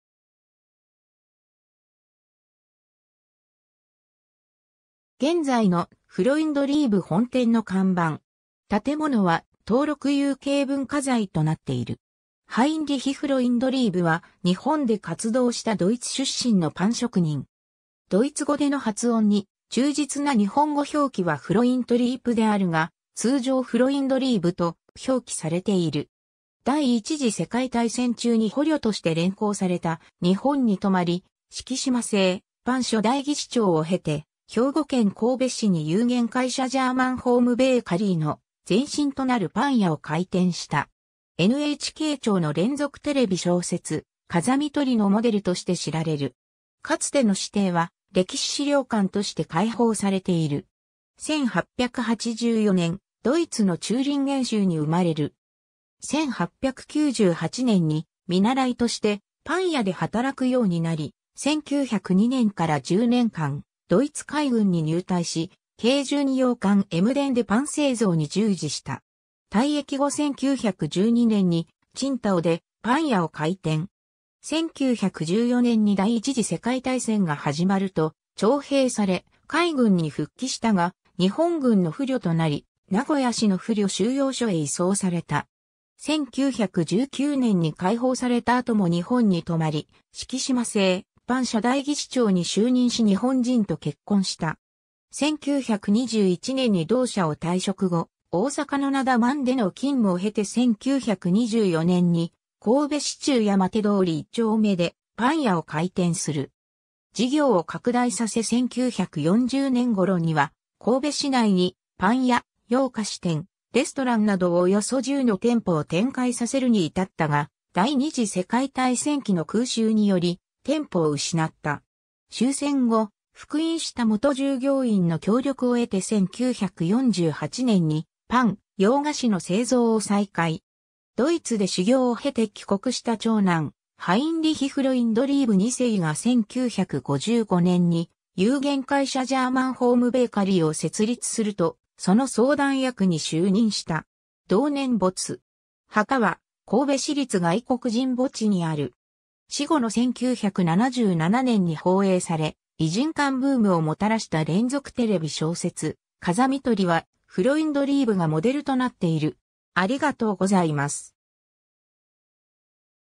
ハインリヒ・フロインドリーブ
現在の「フロインドリーブ」本店の看板。
建物（旧神戸ユニオン教会）は登録有形文化財となっている。
 ハインリヒ・フロインドリーブ（独: Heinrich Freundlieb、1884年 - 1955年）は、日本で活動したドイツ出身のパン職人。
ドイツ語での発音に忠実な日本語表記はフロイントリープであるが、通常フロインドリーブと表記されている。
 第一次世界大戦中に捕虜として連行された日本に留まり、敷島製パン初代技師長を経て兵庫県神戸市に有限会社ジャーマンホームベーカリー（フロインドリーブ）の前身となるパン屋を開店した。
NHK朝の連続テレビ小説『風見鶏』のモデルとして知られる。
 かつての私邸は歴史資料館（北野物語館）として開放されている（1Fにはスターバックス・コーヒーが入居）。
 1884年、ドイツのテューリンゲン州に生まれる。
1898年に見習いとしてパン屋で働くようになり、1902年から10年間ドイツ海軍に入隊し、軽巡洋艦エムデンでパン製造に従事した。
退役後1912年に青島でパン屋を開店。
1914年に第一次世界大戦が始まると徴兵され海軍に復帰したが日本軍の俘虜（捕虜）となり、名古屋市の俘虜収容所へ移送された。
 1919年に解放された後も日本に留まり、敷島製パン初代技師長に就任し日本人と結婚した。
1921年に同社を退職後、 大阪のなだ万での勤務を経て1924年に神戸市中山手通1丁目でパン屋を開店する。
事業を拡大させ1940年頃には神戸市内にパン屋、洋菓子店、レストランなどおよそ10の店舗を展開させるに至ったが、第二次世界大戦期の空襲（神戸大空襲）により店舗を失った。
 終戦後、復員した元従業員の協力を得て1948年にパン・洋菓子の製造を再開。
ドイツで修行を経て帰国した長男ハインリヒ・フロインドリーブ2世が1955年に有限会社ジャーマンホームベーカリー（フロインドリーブ）を設立するとその相談役に就任した。
同年没。
墓は神戸市立外国人墓地にある。
 死後の1977年に放映され異人館ブームをもたらした連続テレビ小説『風見鶏』は、フロインドリーブがモデルとなっている。